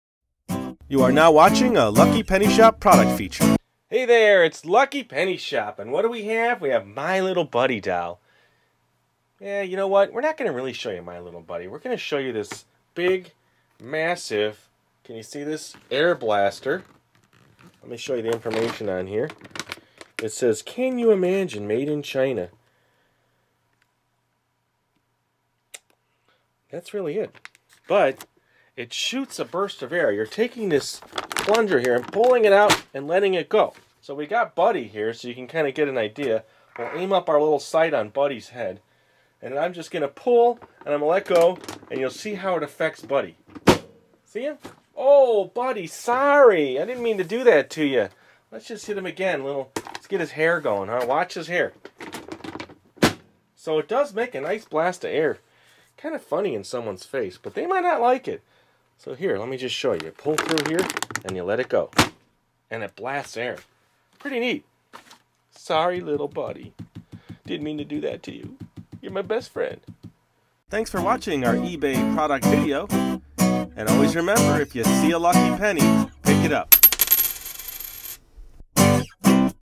Black Air Zooka Air Launcher Cannon Blaster Gun, Can You Imagine Toys
​​​Watch our product feature video for a Black Air Zooka Air Launcher Cannon Blaster Gun, Can You Imagine Toys.